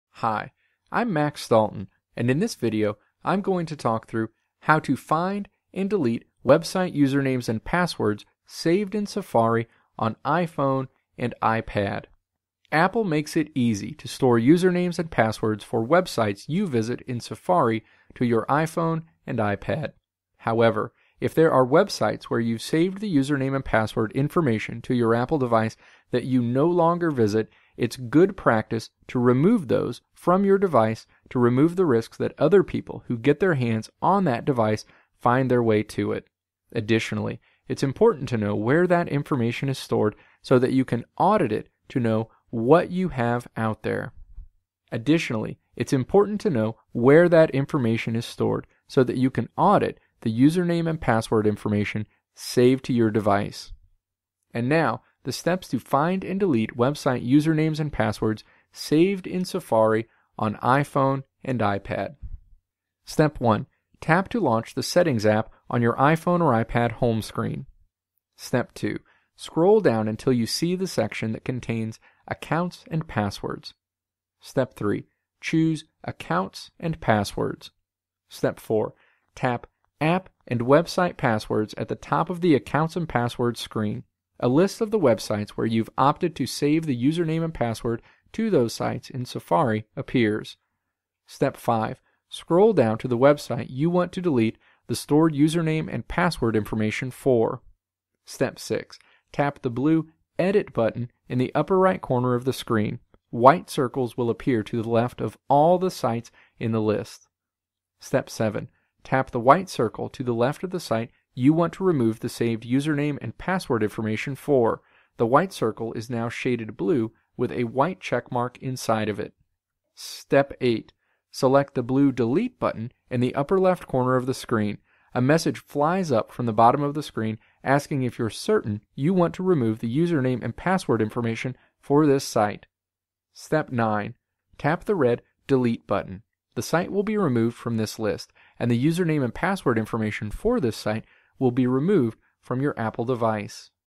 How to Find and Delete Website Username and Password Information Saved in Safari on iPhone or iPad
This video walks you through how to find and delete website username and password information saved in Safari on Apple iPhone and iPad.